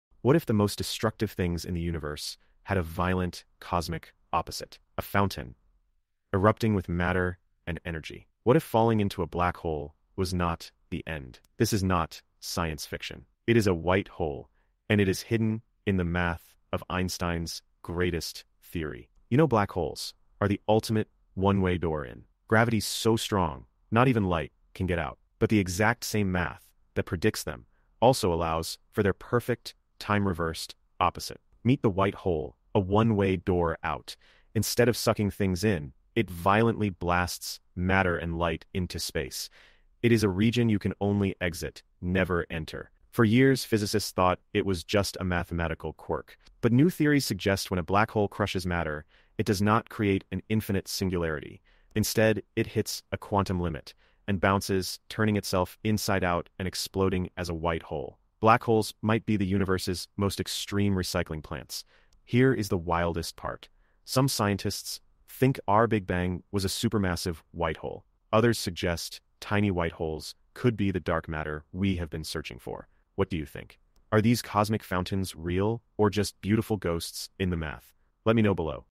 White Holes The Universe's Greatest Mystery
Delve into the fascinating world of white holes, the universe's greatest mystery. Learn about the science behind these enigmatic cosmic phenomena in this video.